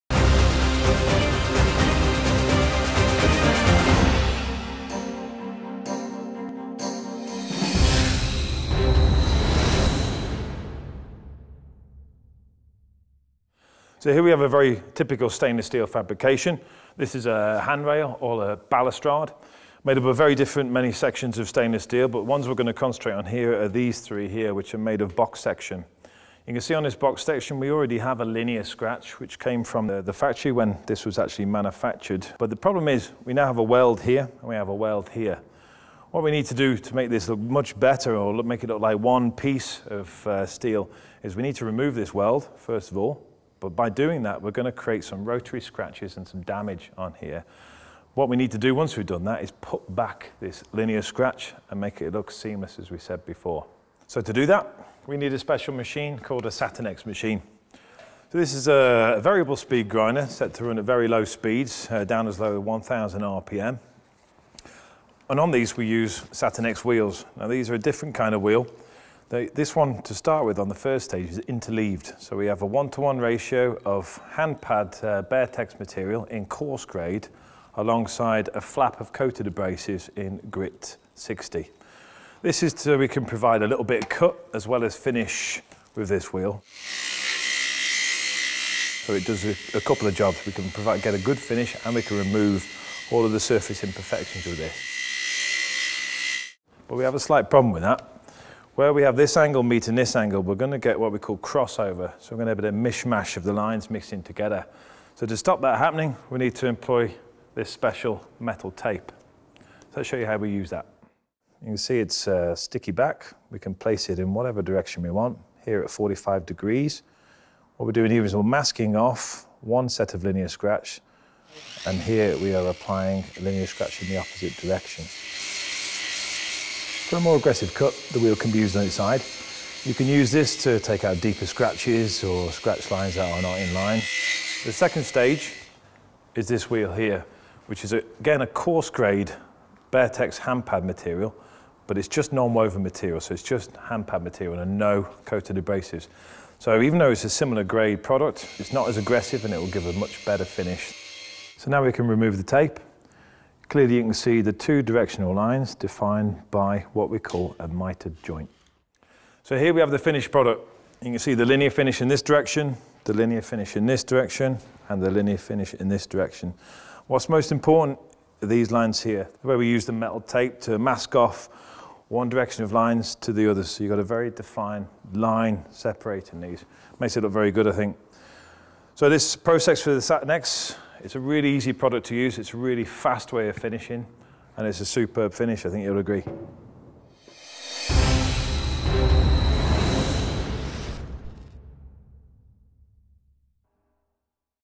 Blending & Finishing large surface areas with BearTex Satinex Wheels
Paul Gray demonstrates how to put a linear scratch back onto large surface areas with Norton BearTex wheels.